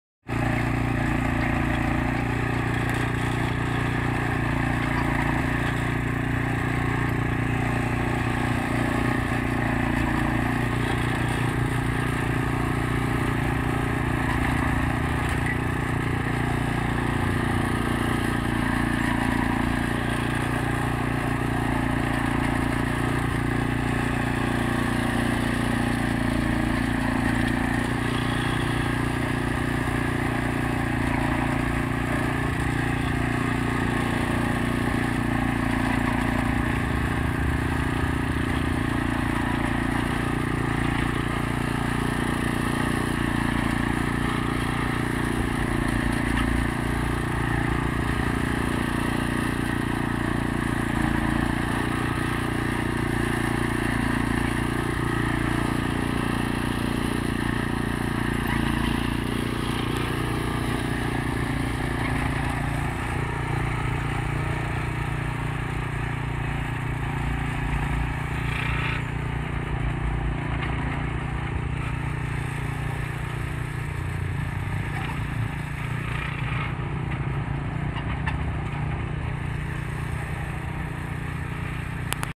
Chequeos de las vias nuevas en el Merlo-Lobos
En este video podran ver que despues de haber sido chequeadas el estado de las vias se las volvia a unir con la maquinaria.
En simultaneo se estaba realizando la soldadura de las vias del Merlo-Lobos de la Linea Sarmiento llegando al PAN Av. San Martin en Marcos Paz.